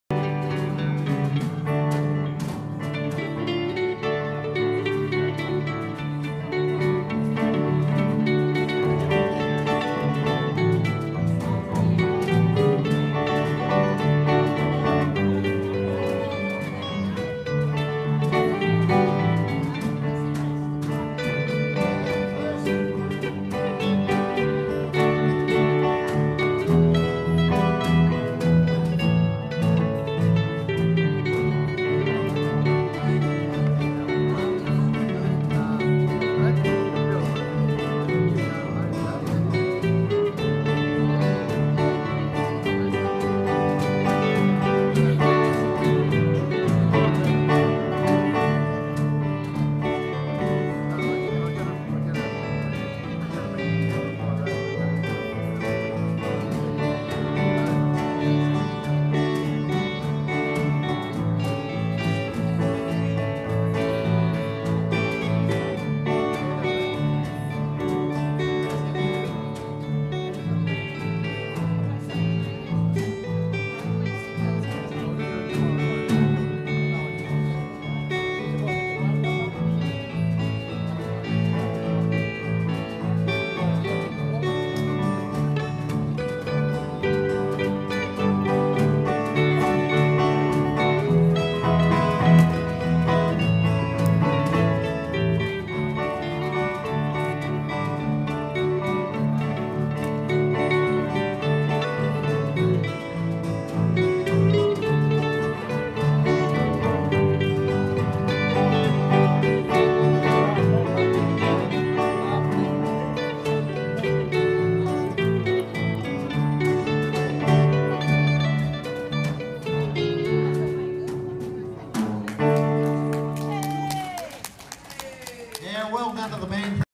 Band Instrumental
The Forbes Band performed at the Forbes February Muster